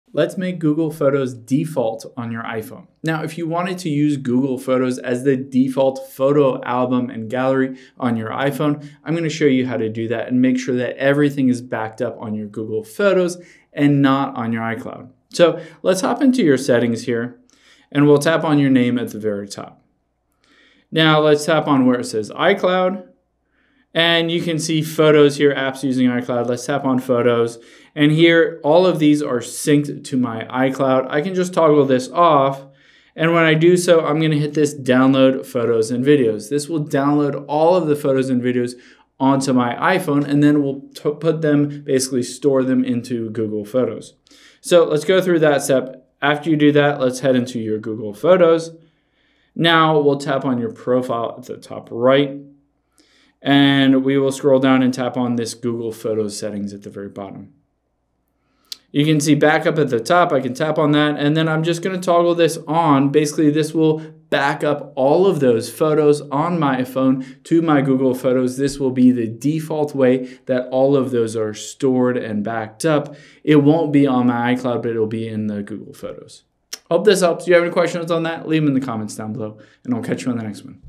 How To Make Google Photos Default On iPhone
Let's make Google Photos the default photos gallery on your iPhone instead of the Apple app.